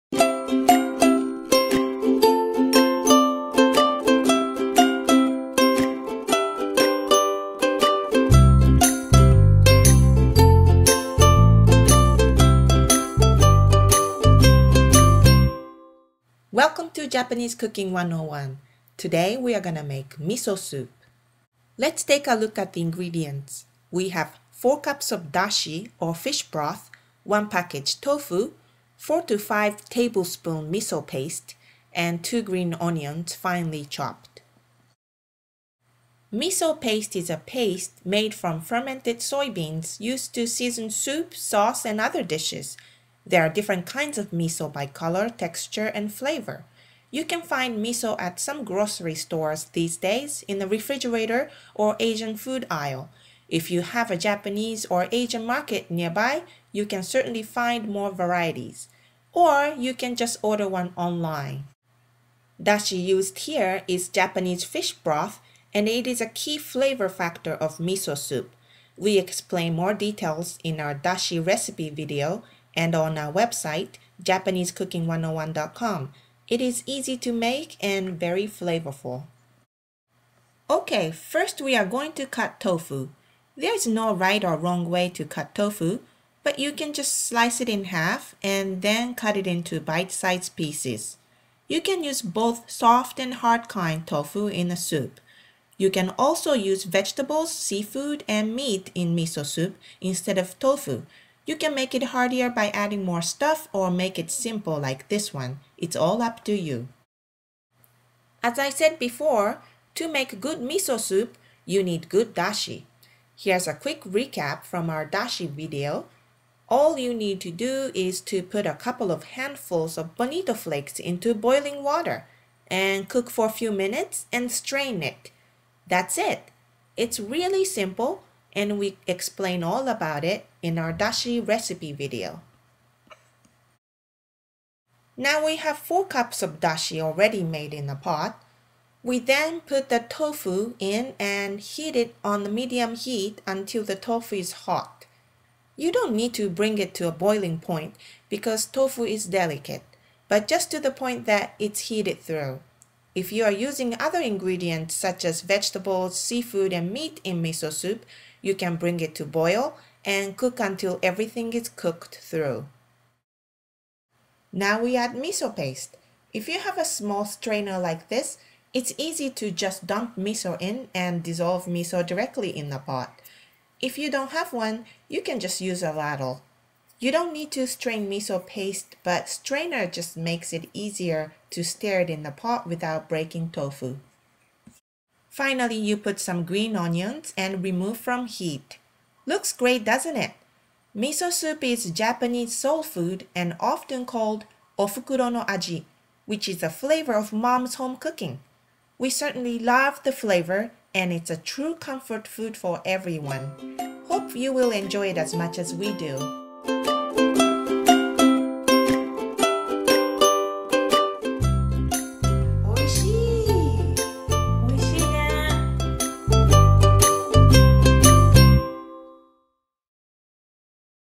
Miso Soup Recipe - Japanese Cooking 101
This video will show you how to make Miso Soup.